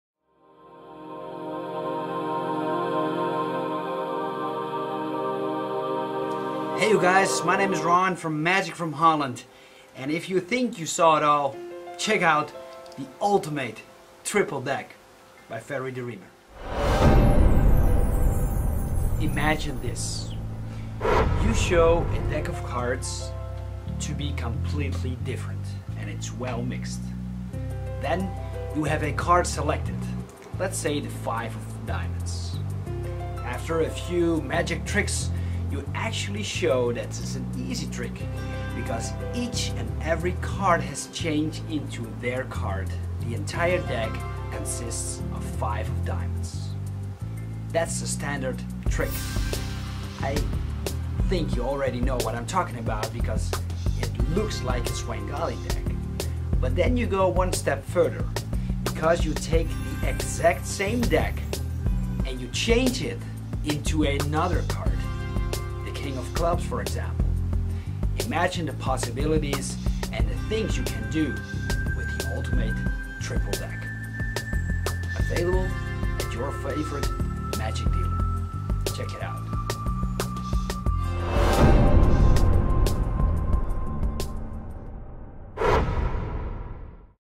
Triple Deck (Ultimate) by Ferry De Riemer
World Magic Shop DO NOT SELL Every product listed anymore. for the items we sell. Quality not Quantity :-) Ultimate Triple deck by Ferry the Riemer & MagicfromHolland

An amazing deck of Bicycle cards that allows you to perform great miracles and are very easy to do!

The deck is based on the famous Svengali deck and you can perform most of the original "Svengali" routines. After you have demonstrated some amazing card skills, you show that the ENTIRE deck has changed into the selected card by spreading the deck face up, so ALL cards are seen!

The audience thinks the trick is over, but than you change the deck back to a well mixed deck of indifferent playing cards. Next you ask a (second) spectator to select another card. This time he will pick ANOTHER card other than the one he just picked. Without any switches, strange moves or what so ever, the ENTIRE deck changes AGAIN. This time it changes into this second selected card.

Yes, you can change the SAME deck 3 times and show it clearly each time!

No sleight of hand required
Instant reset
Professionally made in Bicycle stock
Included are easy to follow video instructions